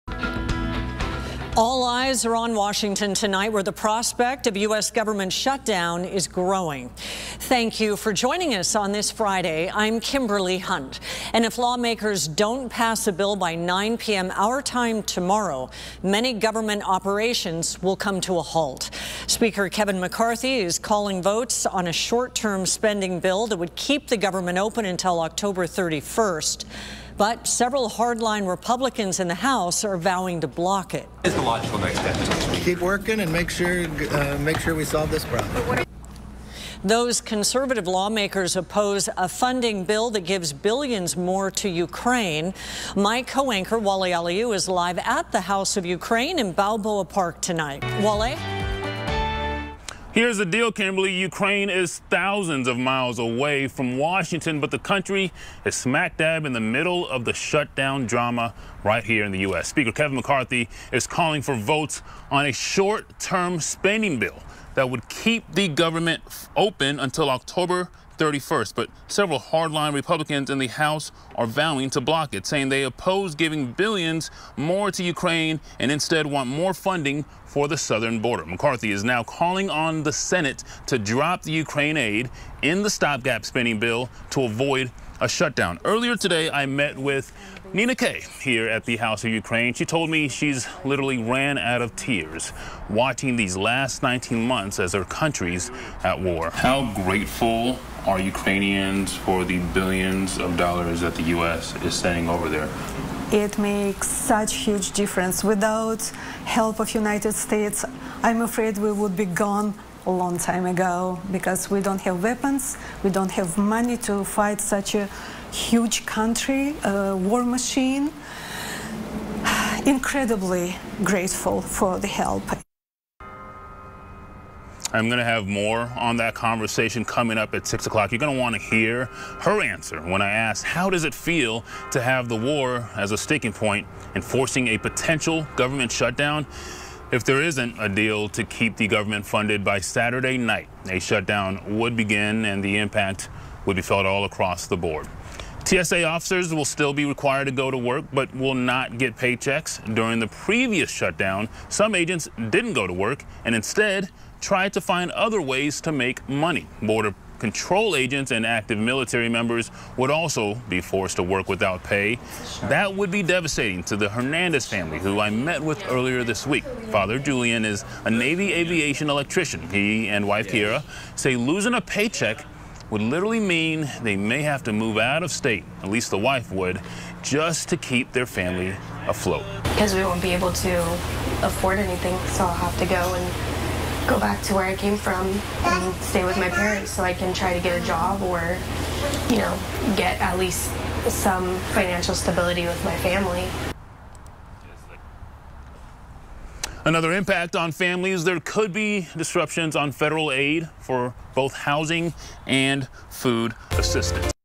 Anchor Wale Aliyu looks into impact of government shutdown
ABC 10News Anchor Wale Aliyu is looking at the government shutdown and how it coul impact local San Diegans.